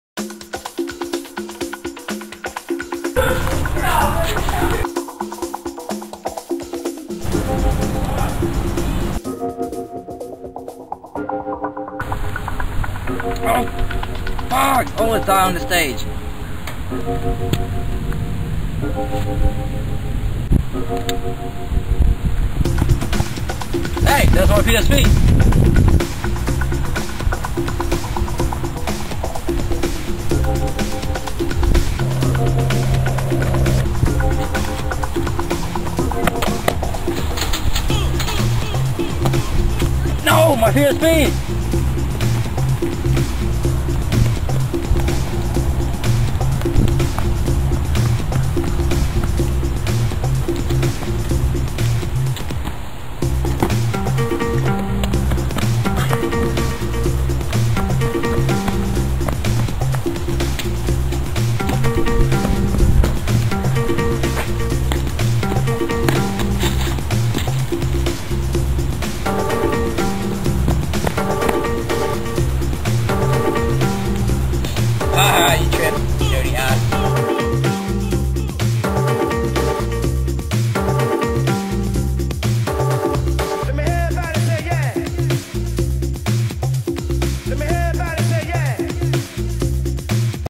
MY PSP!!!
This video is about how Khone got angry and threw his PSP. I come out of no where and snatch his PSP. My attempt to escape by Parkour fails as he gets his PSP back.